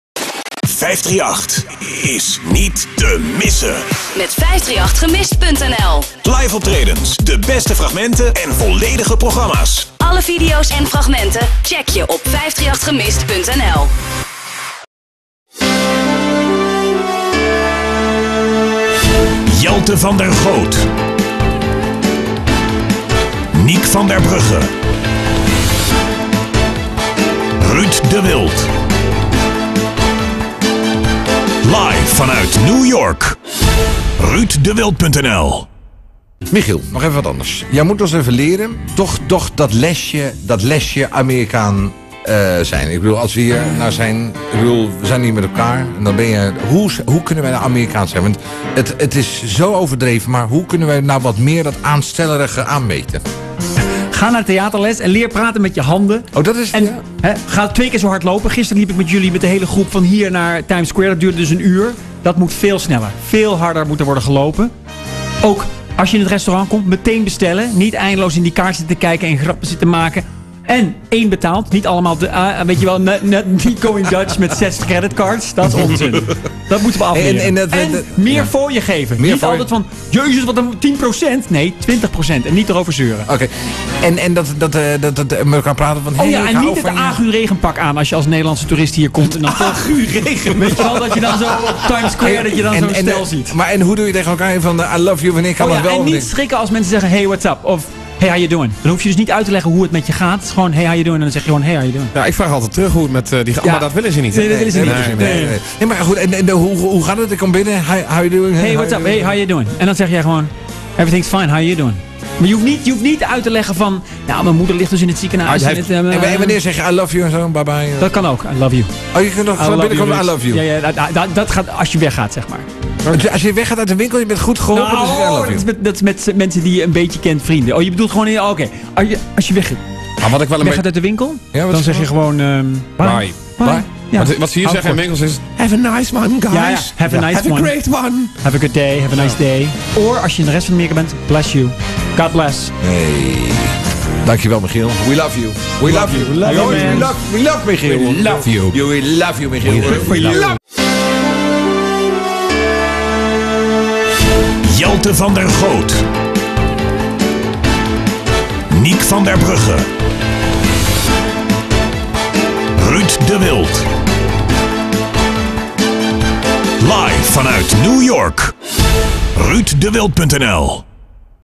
Ruuddewild.nl live vanuit New York: Michiel Vos geeft les over New Yorkers
New Yorkers zijn anders dan de andere inwoners van de USA volgens Michiel Vos. Maar wat maakt ze dan zo anders? Michiel geeft les!